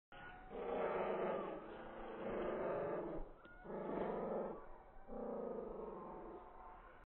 “Is your greyhound bird reactive?”
Jenie demonstrates contempt for a swooping magpie at Ernest Johnson oval, South Perth.